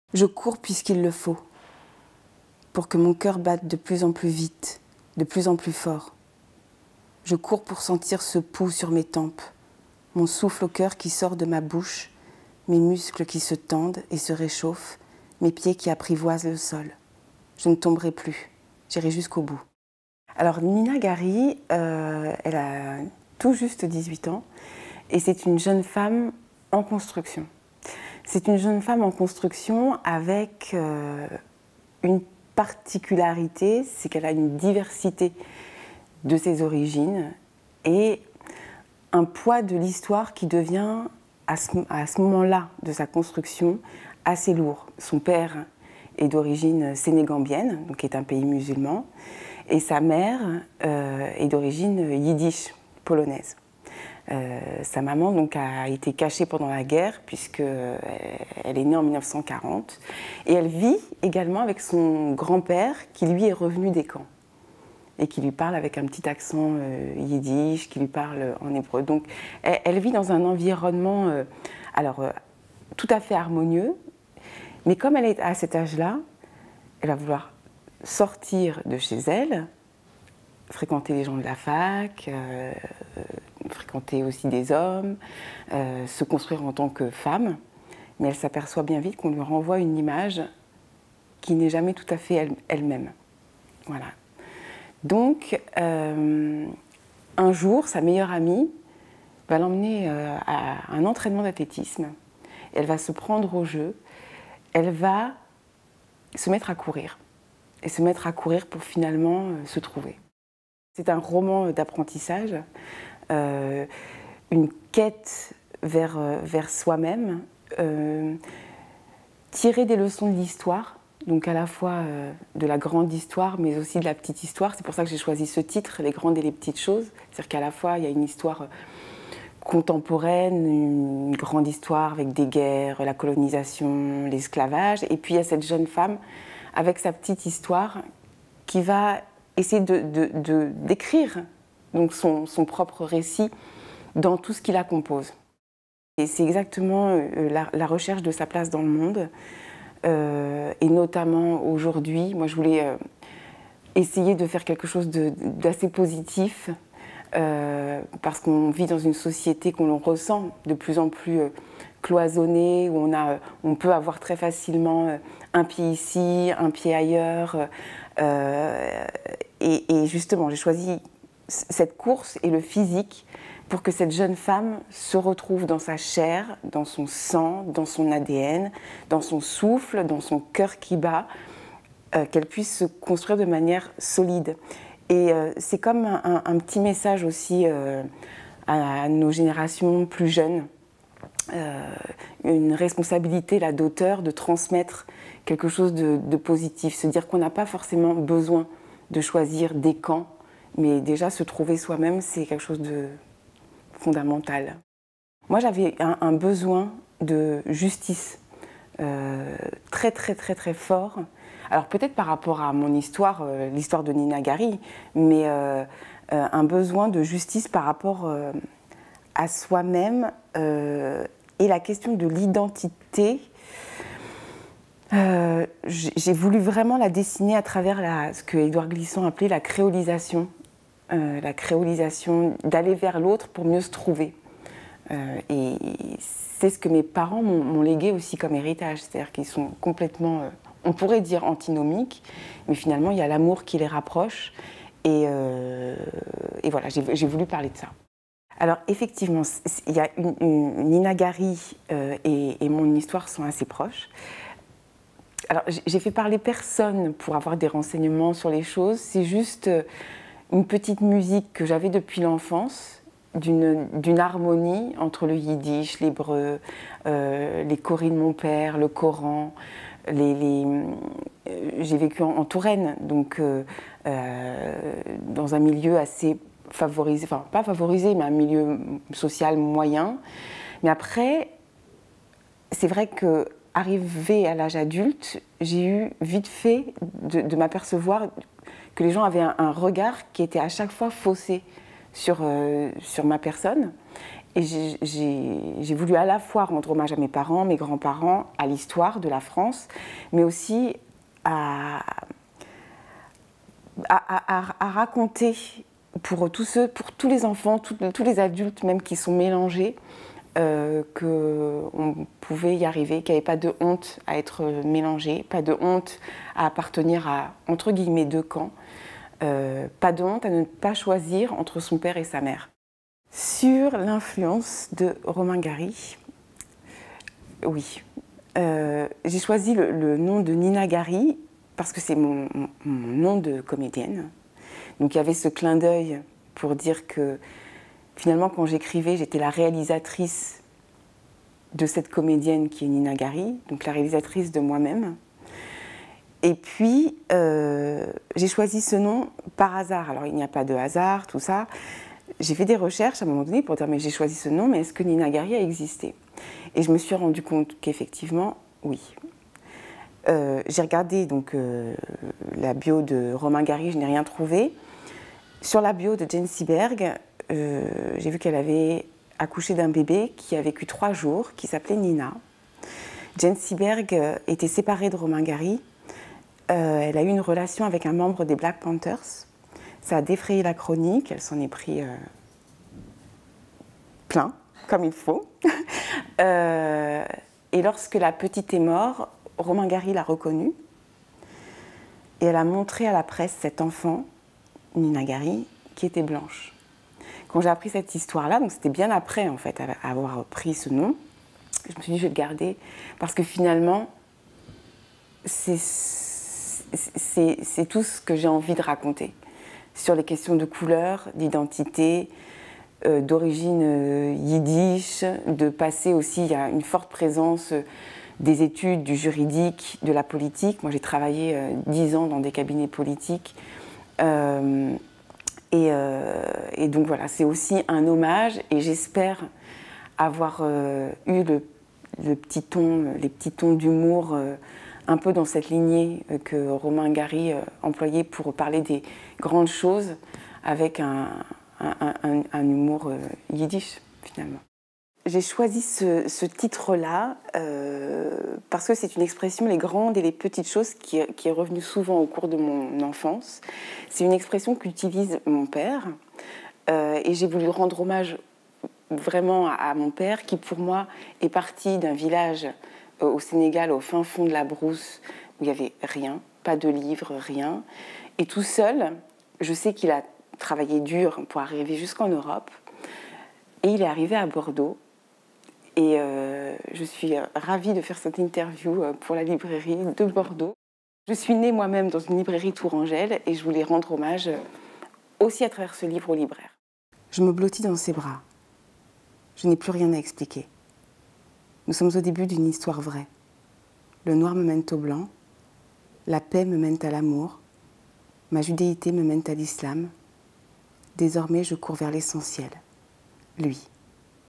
Rachel Khan - Les grandes et les petites choses
Rachel Khan vous présente son ouvrage "Les grandes et les petites choses" aux éditions Anne Carrière. Rentrée littéraire janvier 2016.